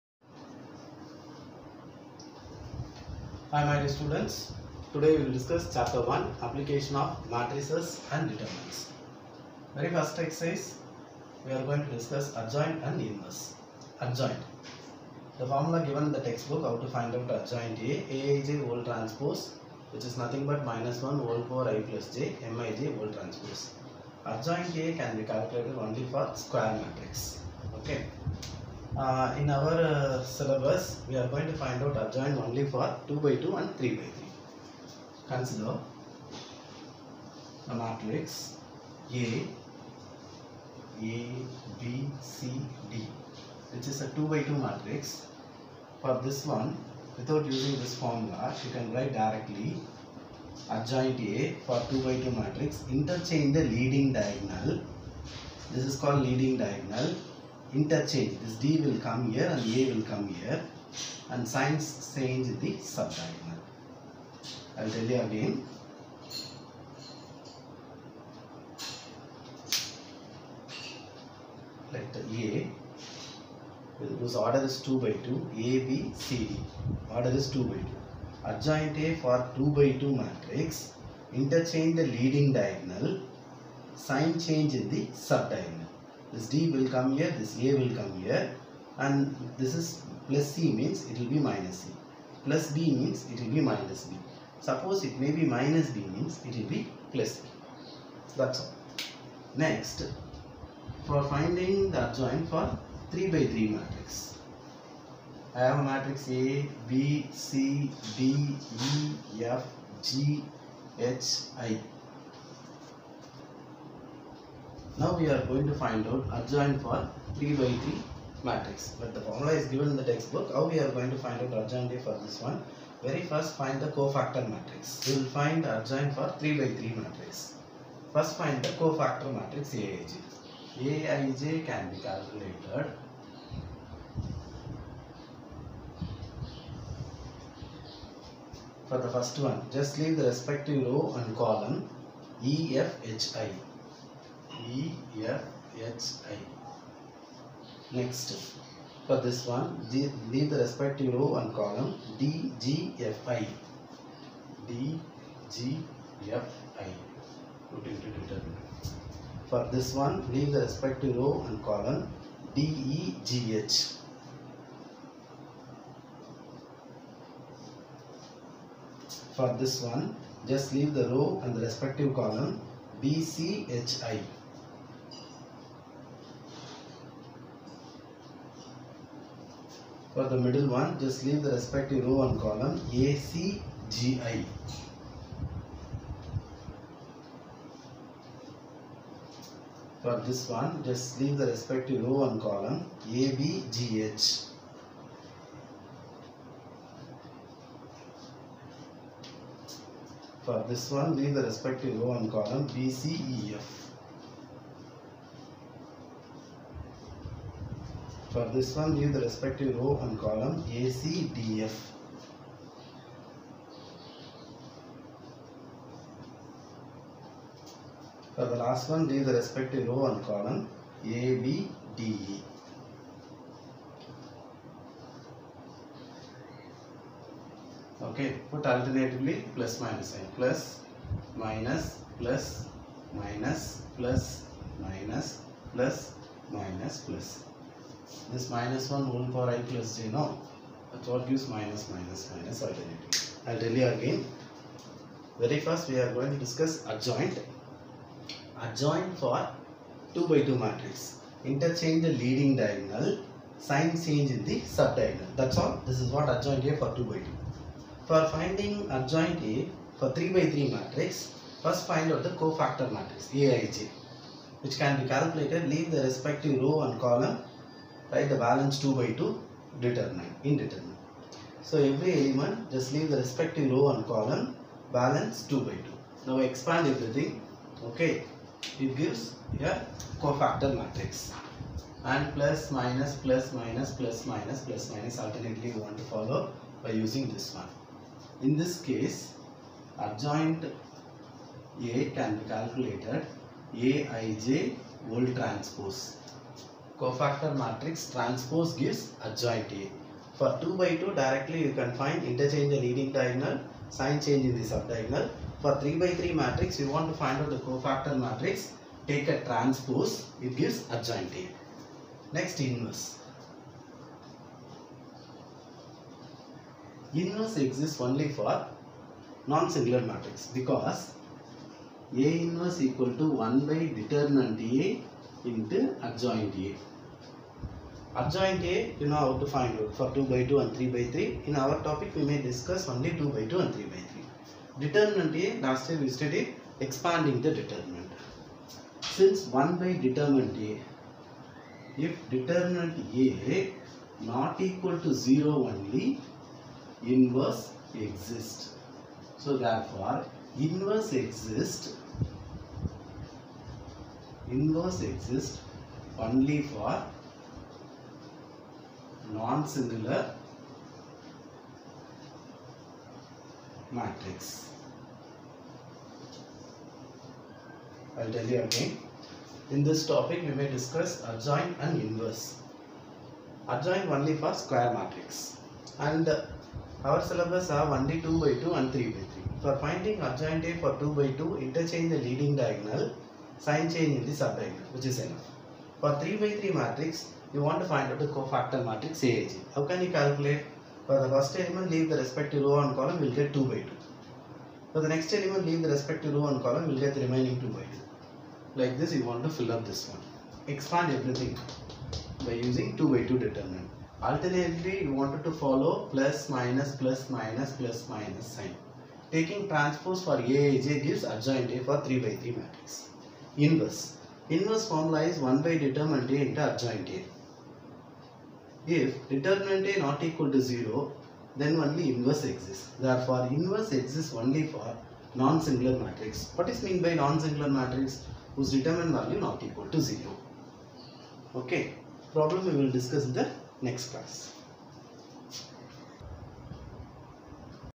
TN (general) Maths text book 12 th standard. Introduction to adjoint and inverse.
In this  video we may discuss how to find out  adjoint for 2×2 and 3×3 matrices. Formula for inverse of a non-singular square matrix.